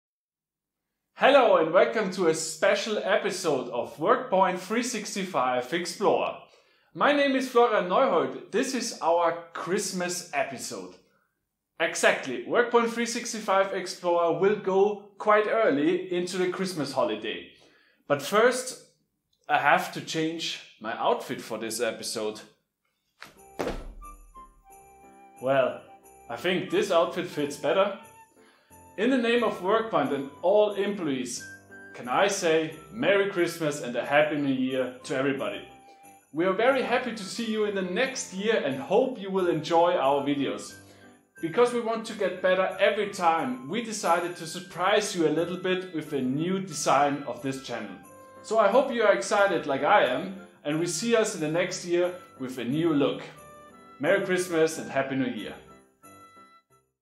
Christmas Episode w Santa Neuhold
WorkPoint 365 Explore will go into Christmas Holiday´s!!
Santa Neuhold wanted to say Merry Christmas and had also a little surprise for you!!